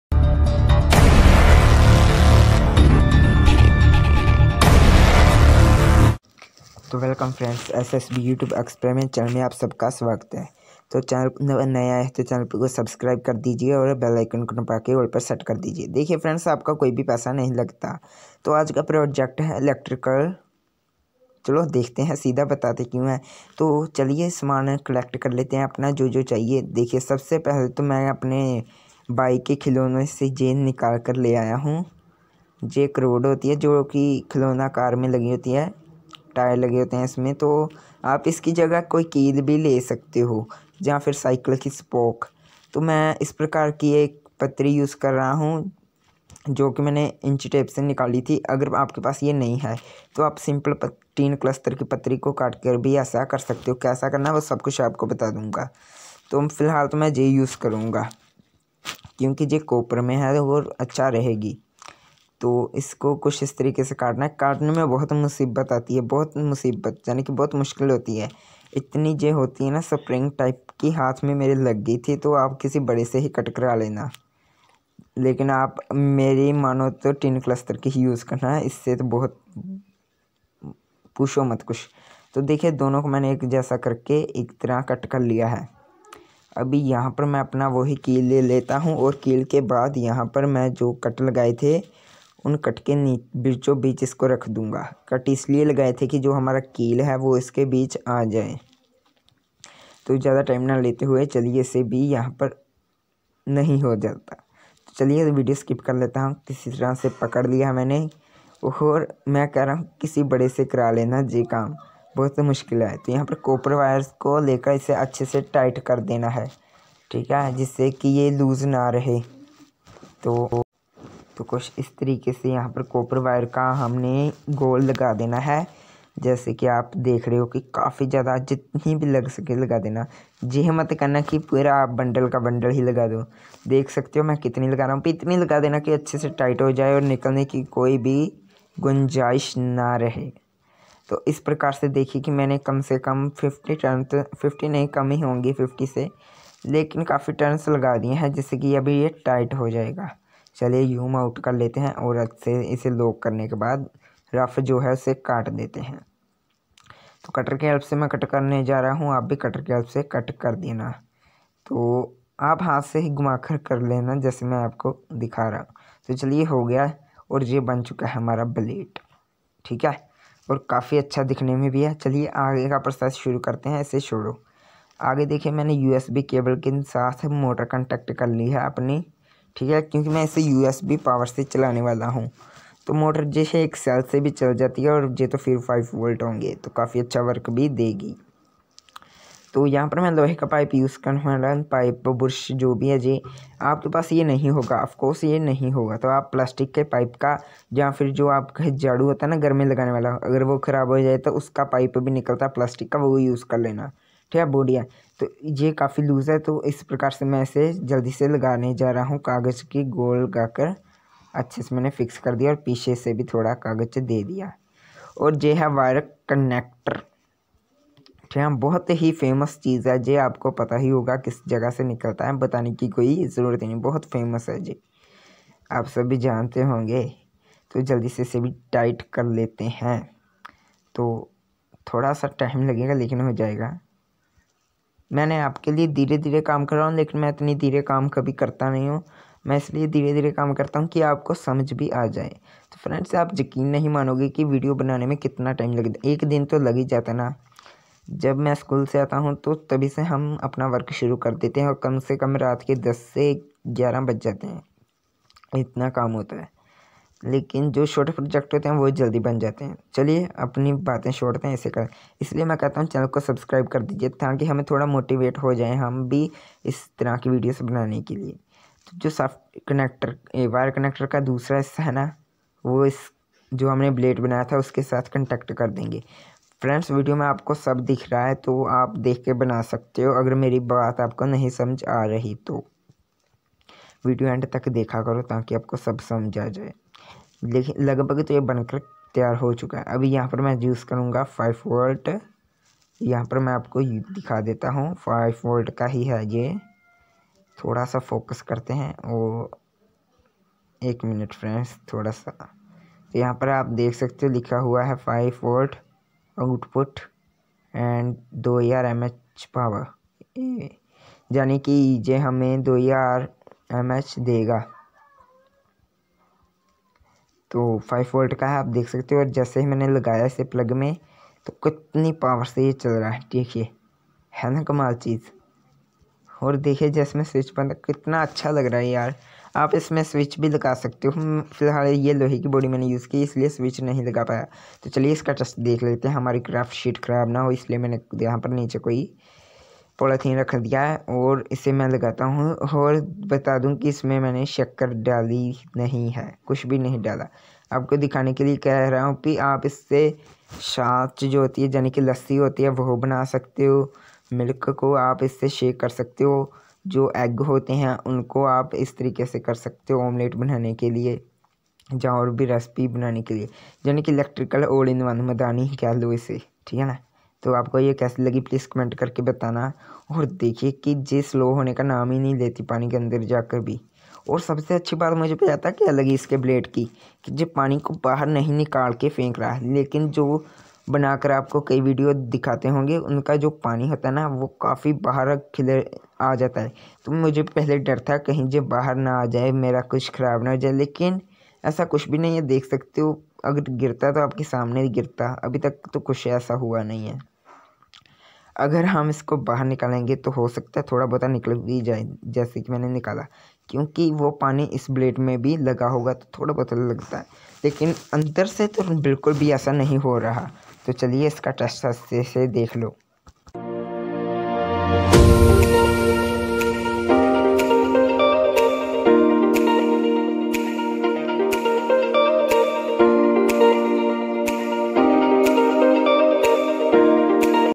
how to make electrical bilinder
hello friends welcome SSB YouTube experiment channel mein aap sabka Swagat hai aaj ki video mein Ham Ek Madani Banane Ja Rahe Hain Jo ki electrical hai aur apne mobile charger se Chalti Hai Agar Jay Pasand aati hai to like kar dena Chandra ko subscribe kar dena Tu video Ke Niche comment kar dena ki kaisi lagi Tu Chali Kisi Aur Ne video mein Milte Hain